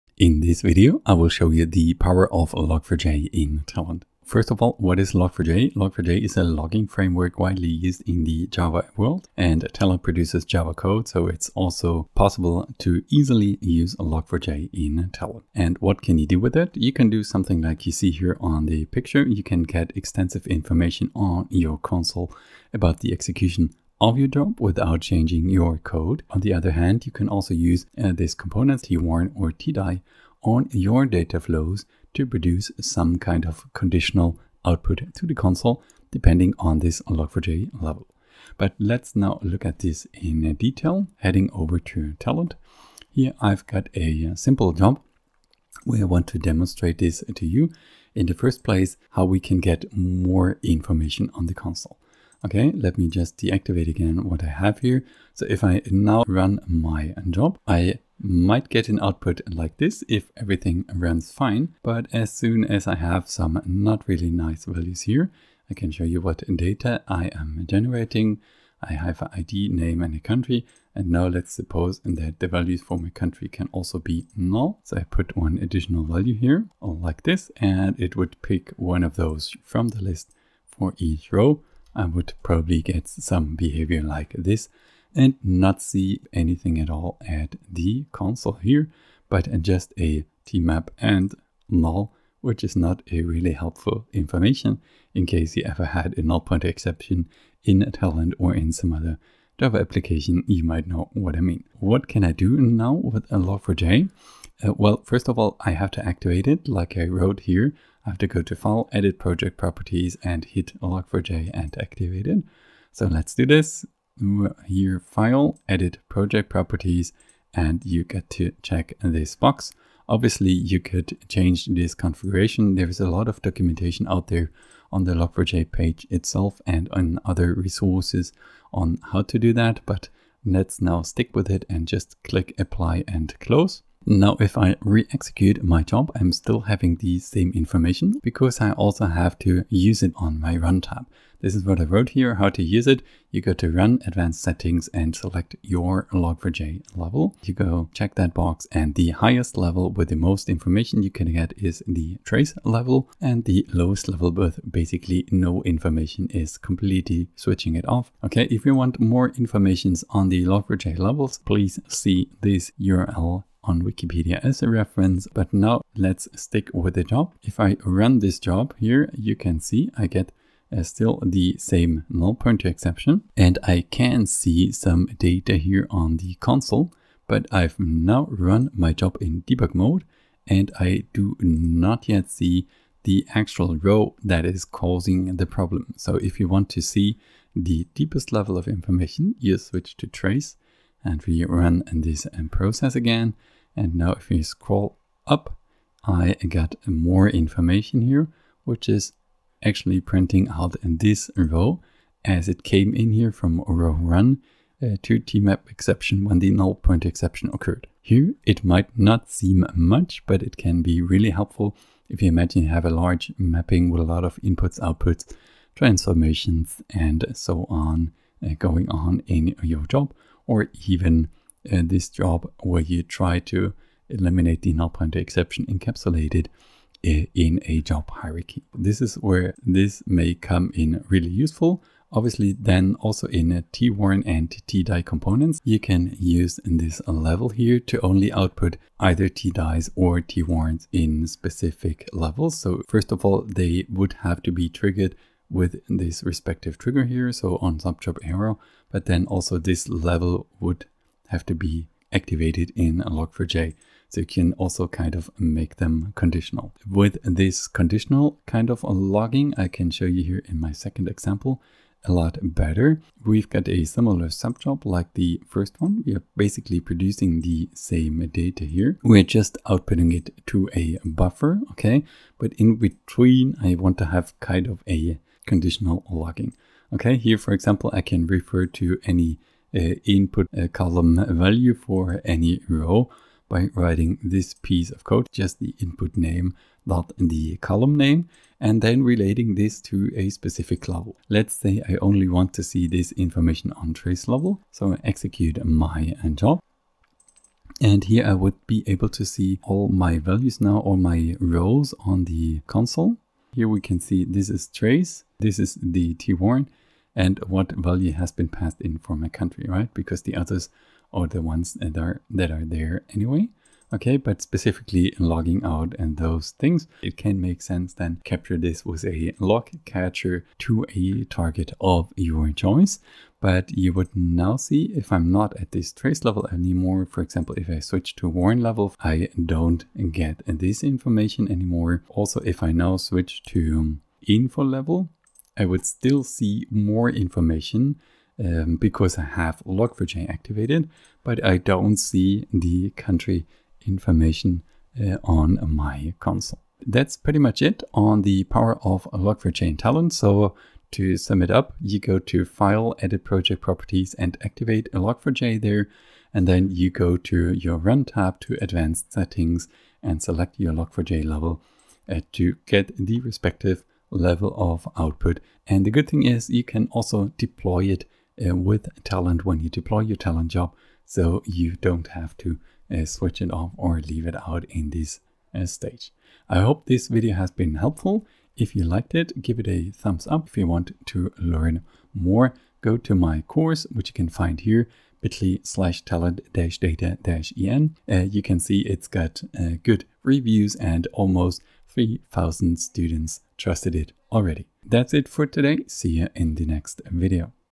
The Power of Log4j in Talend - withour changing your code!
Get (much) more detailed and even conditional output for your jobs whenever you need it, using Log4J in Talend Open Studio.